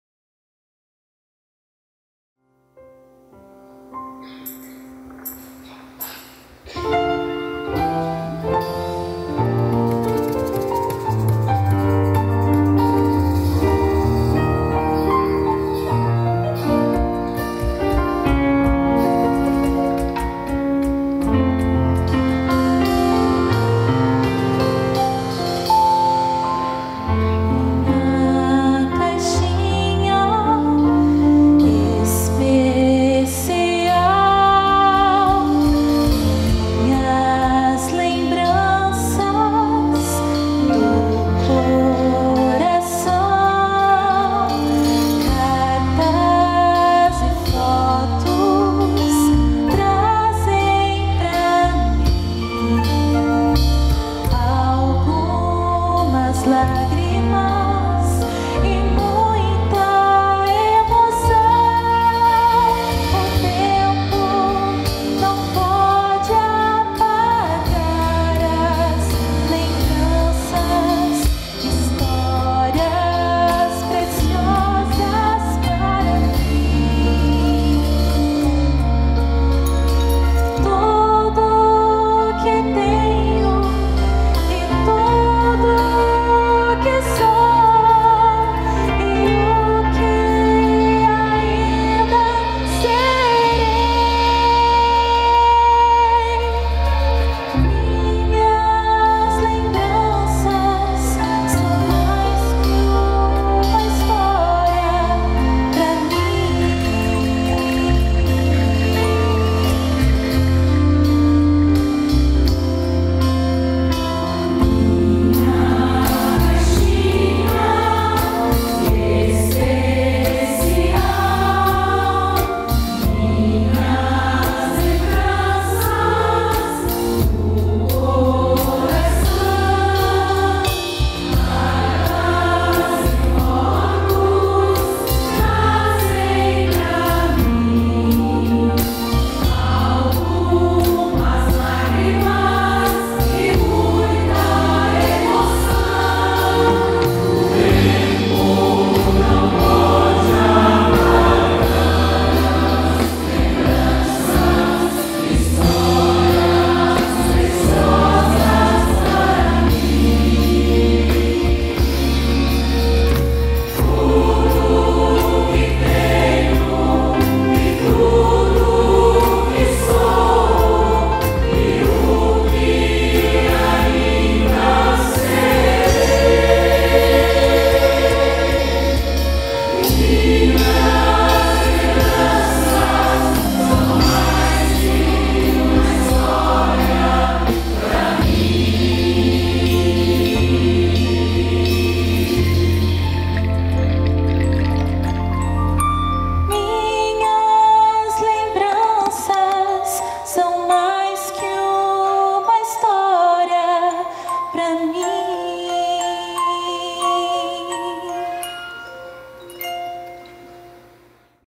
Coral Copel Cascavel - Regente: Nelson Engelmann - Lembranças 1
Coral Copel Cascavel
Regente: Nelson Engelmann
Pocket Show RECORDAR

Evento realizado  no Teatro Municipal de Cascavel Pr, em comemoração aos 61 anos da Copel - Cia Paranaense de Energia.
O Espetáculo tem duração de 1 hora com repertório nacional e internacional. O Pocket Show Recordar conta a história da personagem Milu com sua lembranças e fatos que marcaram sua vida.
Regente: Nelson Engelmann
Pianista:  Marcos Arthur Fitzthum e teclados Róger Borges Araujo.
Percussão: João Aberto Engelmann, Eddy W. Leger e Cesar Augusto de Lima.
Coordenador do Coral: Ricardo Gomes Quadros
Coordenador Geral do Corais da Copel: Andre da Silva e Cênica: Bia Bianco
Aos maestros dos Corais da Copel PR: Erico, Cristiano, Maria Goretti e Rodrigo. Obrigado e valeu pela parceria!
"Deixe aflorar seus sentimentos e recordações mais profundas... minhas lembranças são mais que uma história pra mim!"